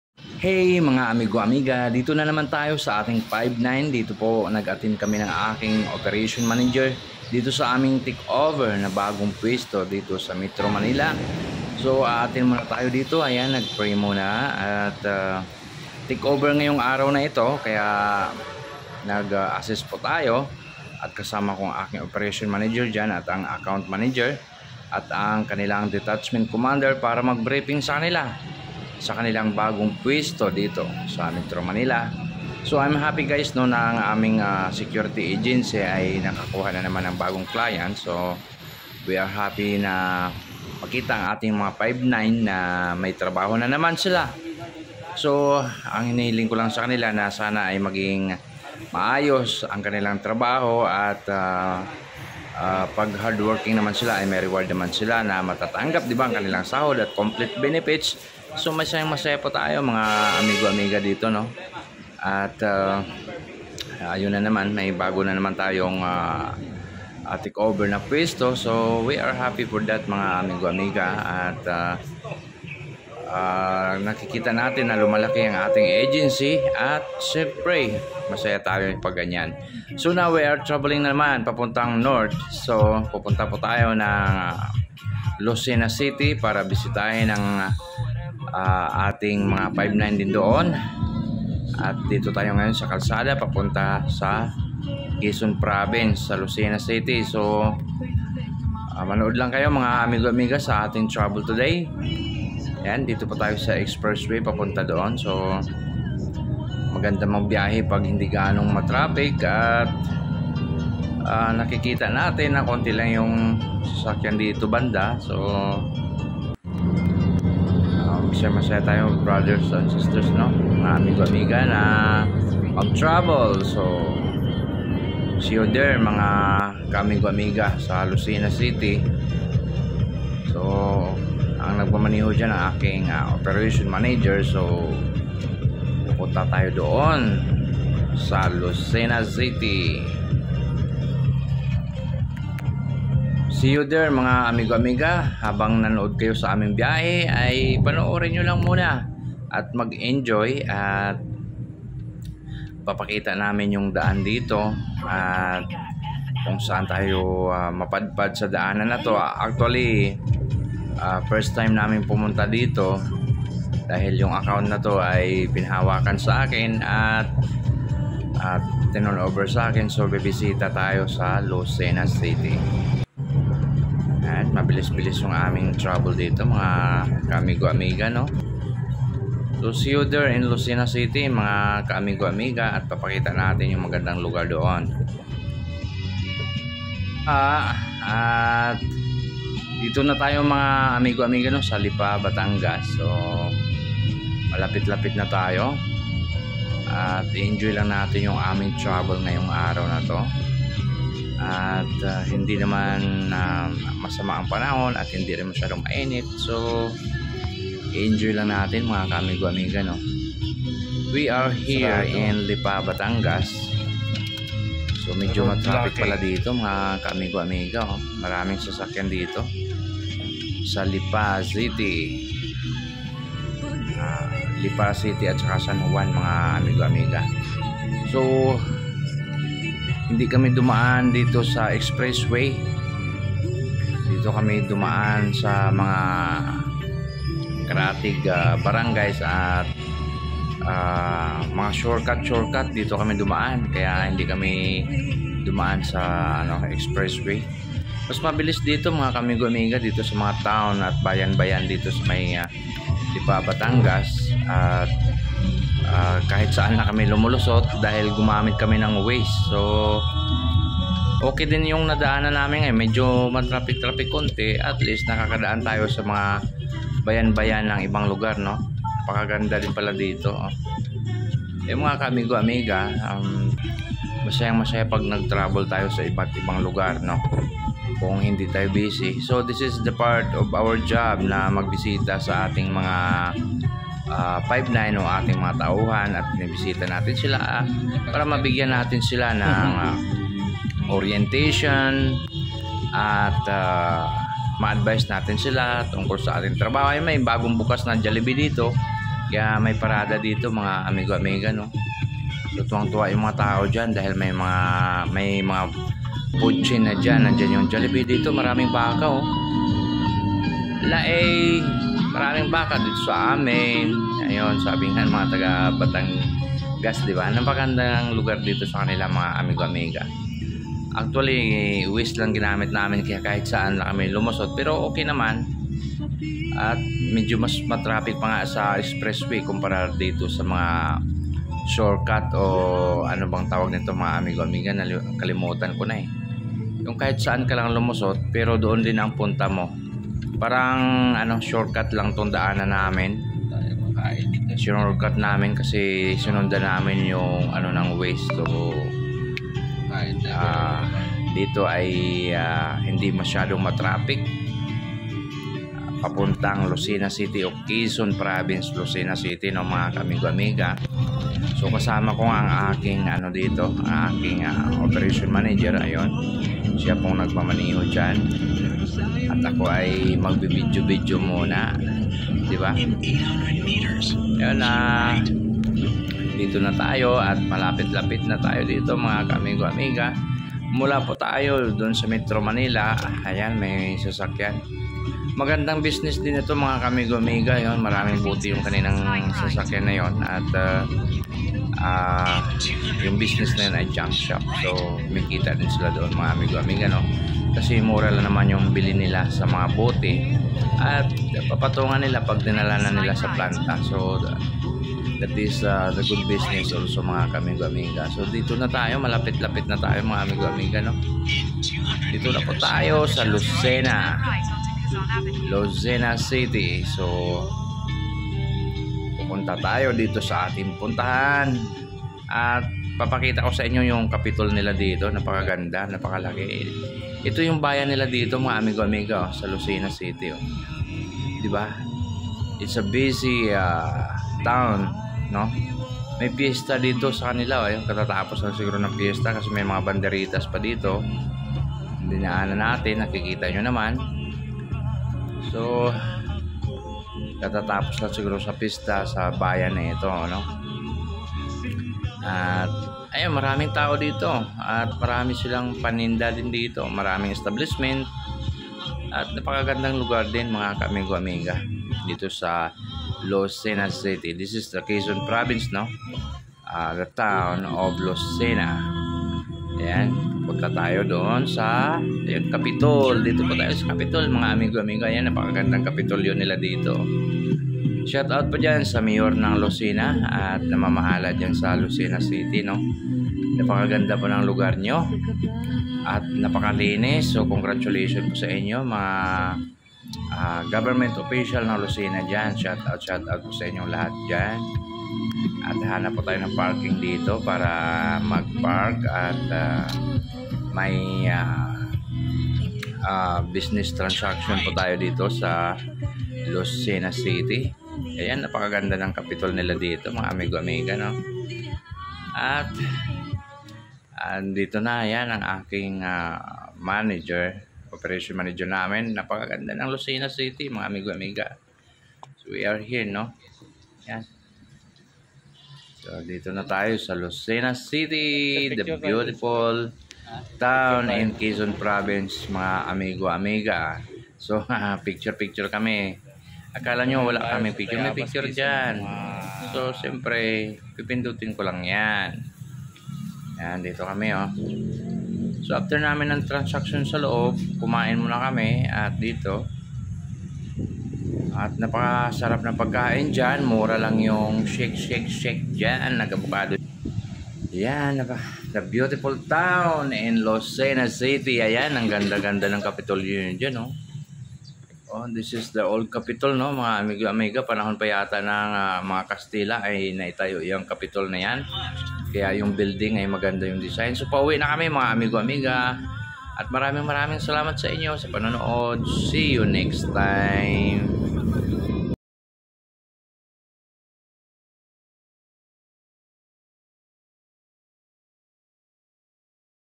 Orientation and Visited to our 59| Malunes j
assist and Orientation to our men for the new posting and visit at Lucena city.